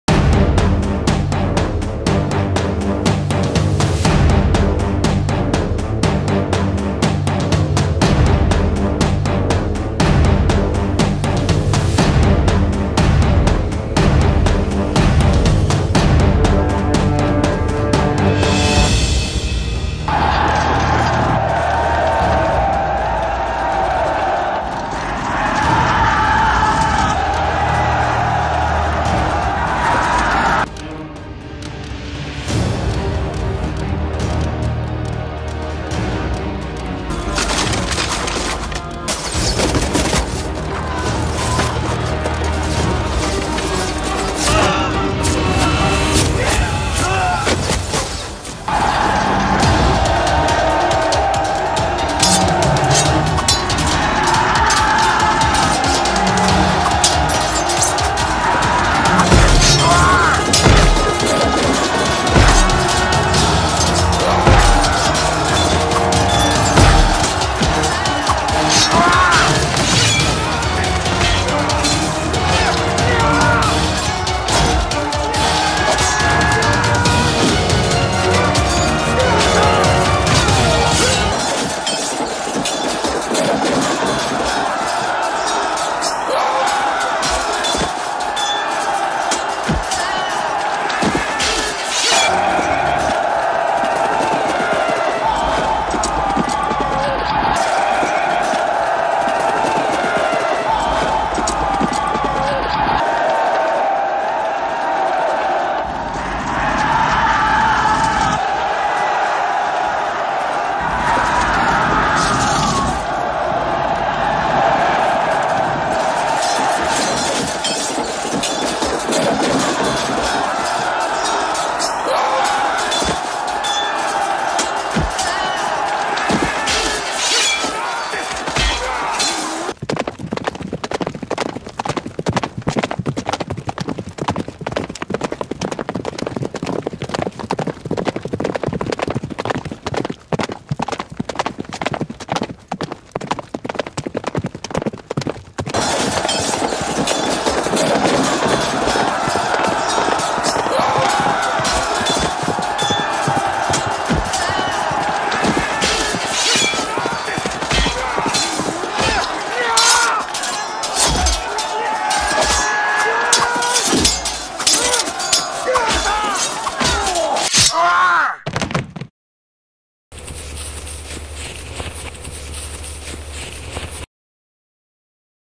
Mongols invasion into Europe / Medieval battle / Lego film (animation)
Thank you World of Warships for supporting my channel this month. to get credits and premium items.

My new animation about Mongol tribes invaded Europe and a battle between Europe knights and Mongols